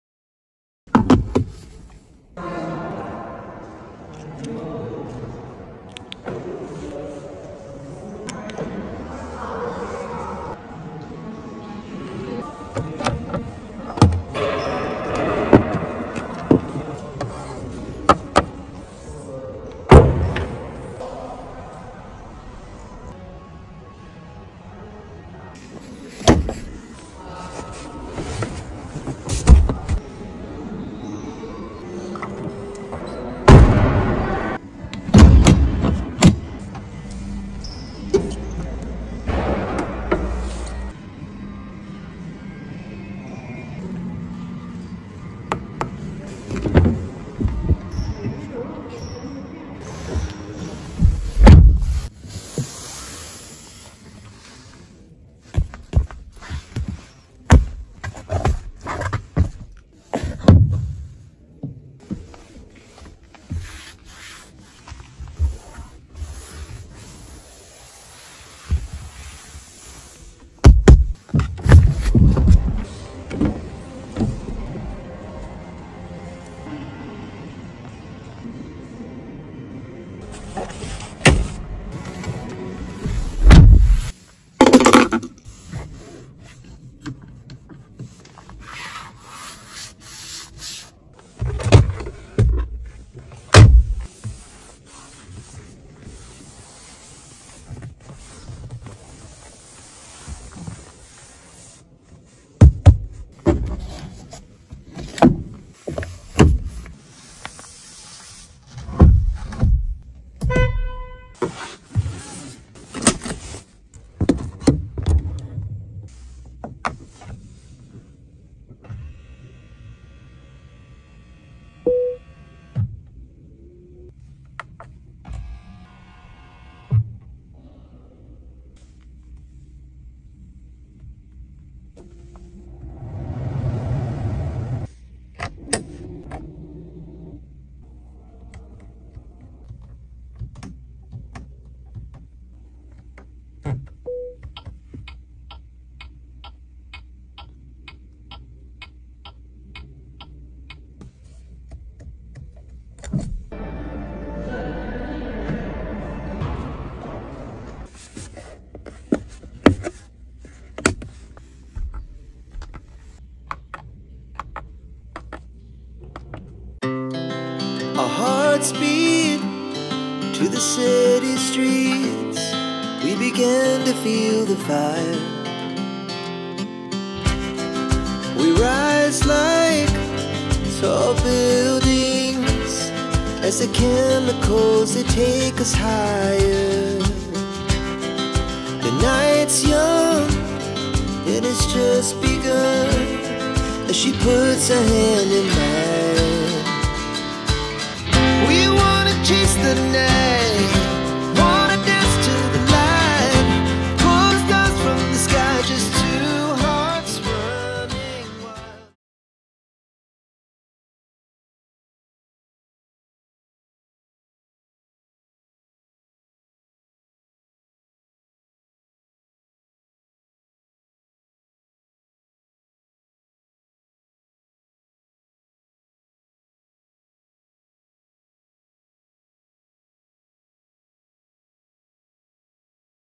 2023 BYD Seagull in-depth Walkaround
2023 BYD Seagull in-depth Walkaround

The 2023 BYD Seagull is equipped with a powertrain that delivers 75 horsepower (55 kW) and a torque of 135 Nm. This small, pure electric car boasts a top speed of 130 km/h, showcasing its performance capabilities. Accelerating from 0 to 50 km/h in just 4.9 seconds, the Seagull demonstrates impressive agility.

In terms of its electric capabilities, the BYD Seagull offers a notable battery range of 305 km. The fast charge time is remarkably efficient, requiring only 0.5 hours, while the slow charge time from 30% to 80% capacity takes 30 minutes. The car's curb weight ranges from 1160 to 1460 kg, contributing to its overall efficiency and maneuverability.

The dimensions of the BYD Seagull are measured at 3780 x 1715 x 1540 mm (length x width x height), with a wheelbase of 2500 mm. The car features left-hand drive (LHD) steering and has a 5-door, 4-seater body configuration, providing practicality and comfort for its occupants.

Notably, the BYD Seagull incorporates LFP Blade battery technology, enhancing its energy storage capabilities. As part of BYD's Ocean series, this front-wheel-drive electric car not only emphasizes performance but also aligns with eco-friendly and sustainable driving practices. Recharge options include a household AC power plug at 6.6 kW and a DC fast charger at 30 or 40 kW, offering versatility in charging preferences for users.